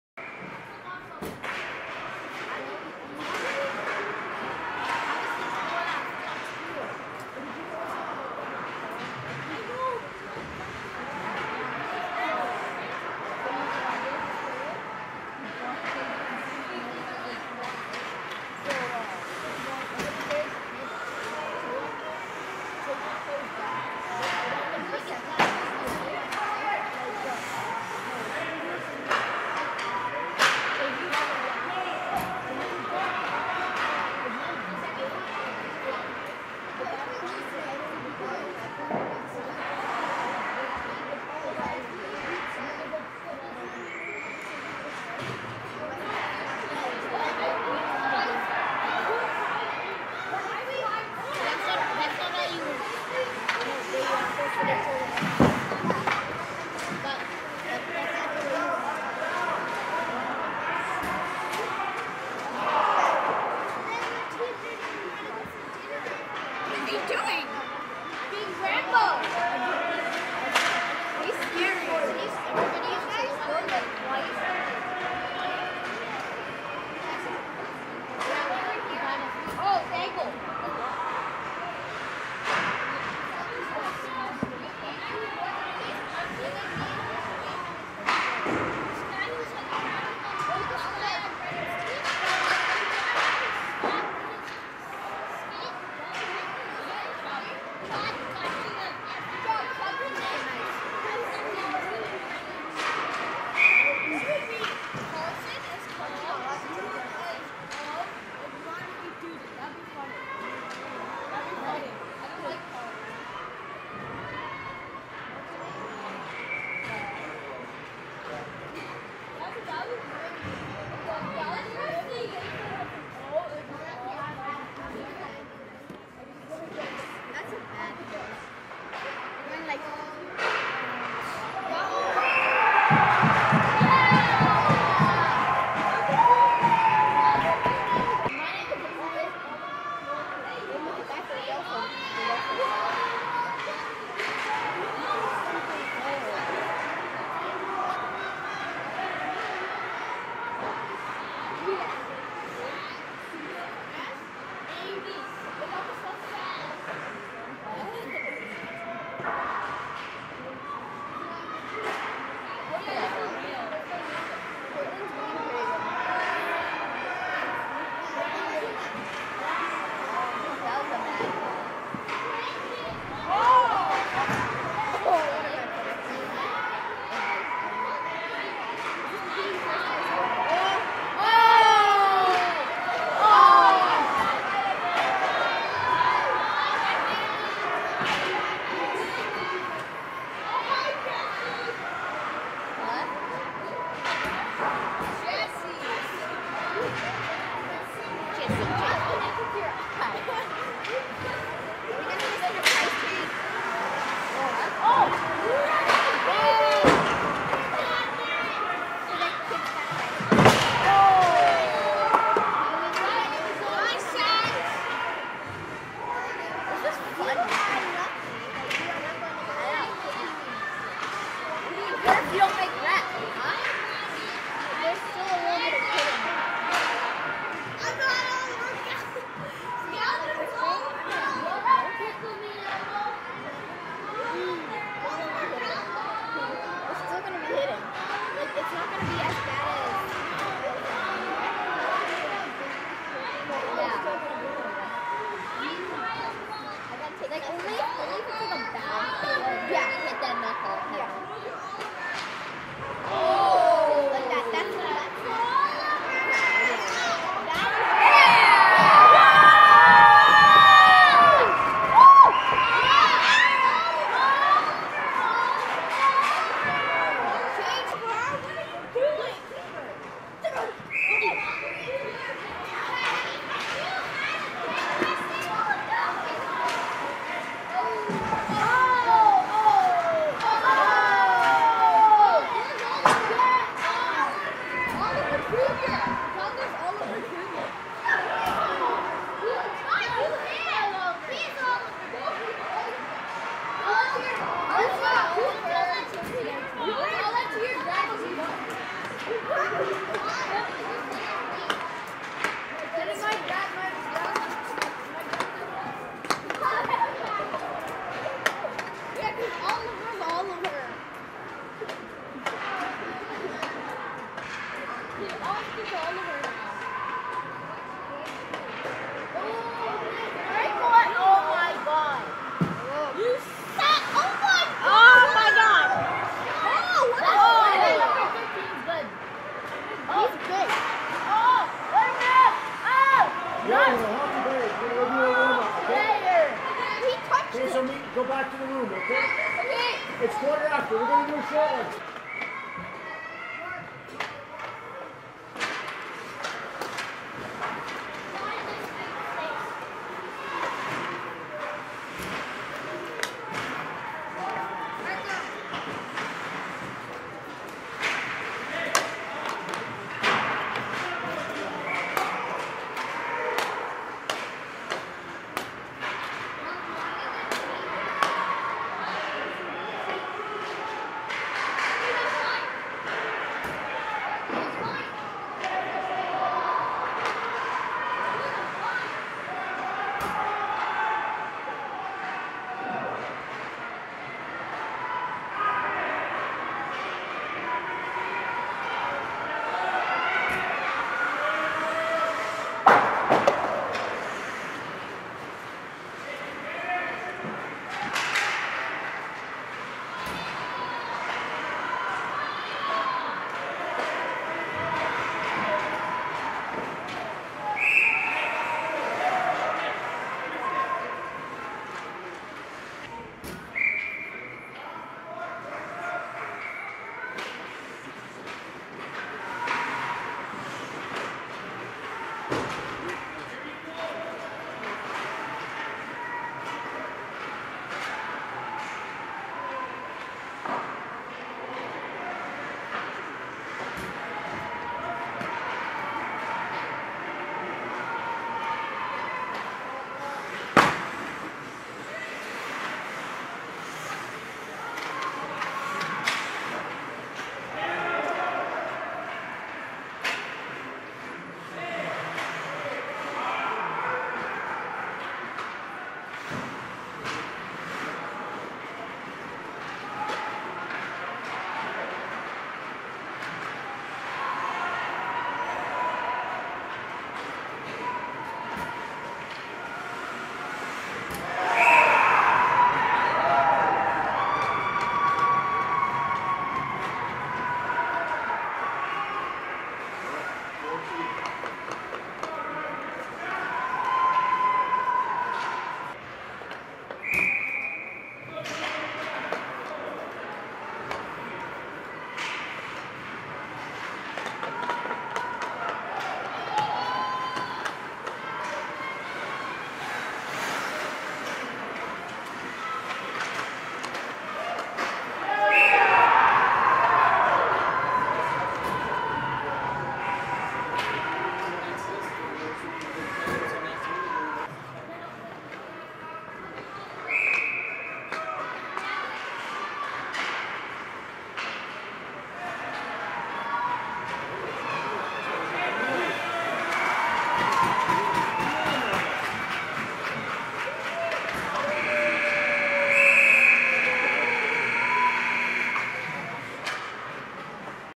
03-17-2013 1st Period Part 2 PJHU12AA vs Vancouver T Birds A4
Game 4 Round Robin of the 2013 Richmond Spring Classic Hockey Tournament. Game played at Richmond Ice Centre in Richmond BC Find me at: